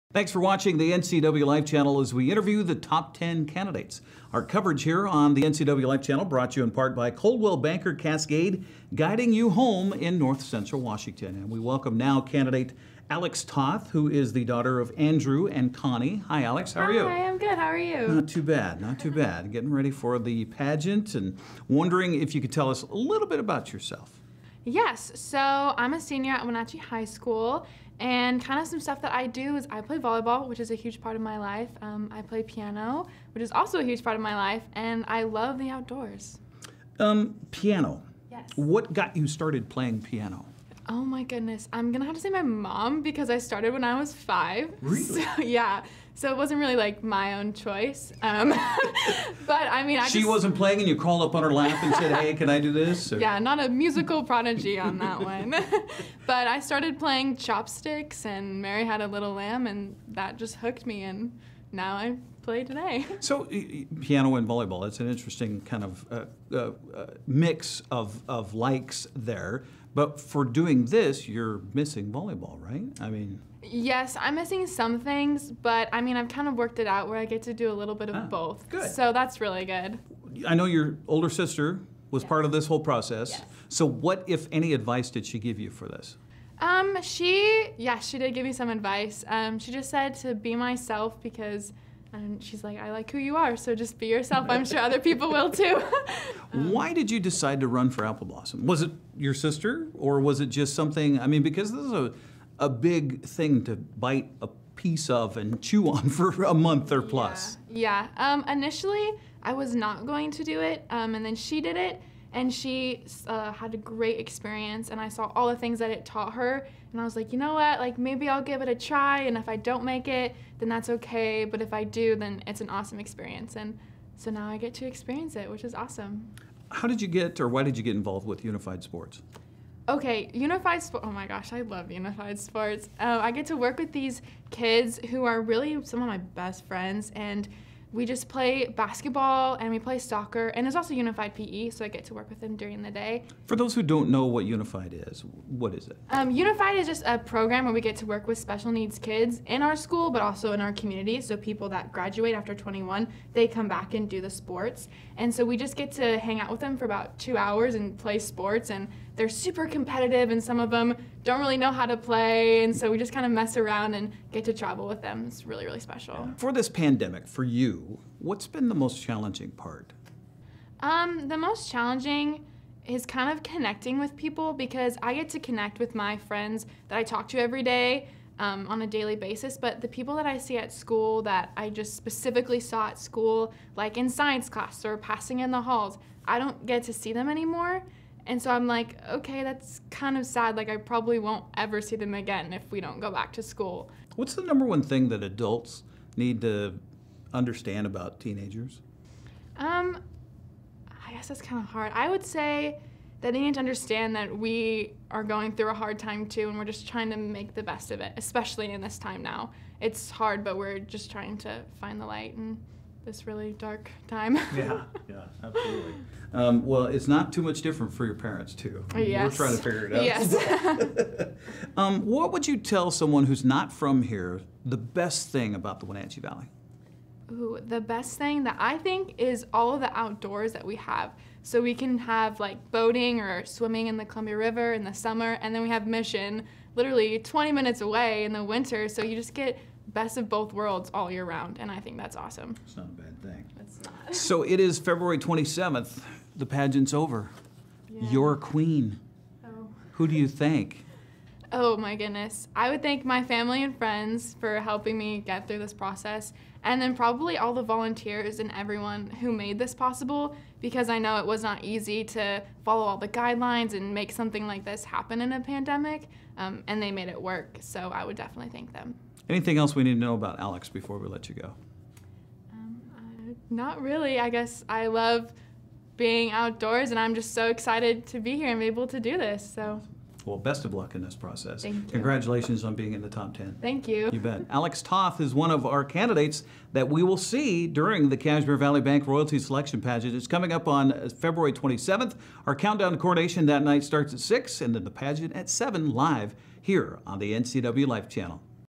Apple Blossom Candidate Alex Toth
Top 10 Candidate Alex Toth is the daughter of Andrew and Connie Toth.  Alex joined NCWLIFE Channel's Eric Granstrom for an interview prior to the Cashmere Valley Bank Royalty Selection Pageant.  Watch for the Countdown to Coronation Saturday, February 27th at 6pm followed by the pageant LIVE at 7pm.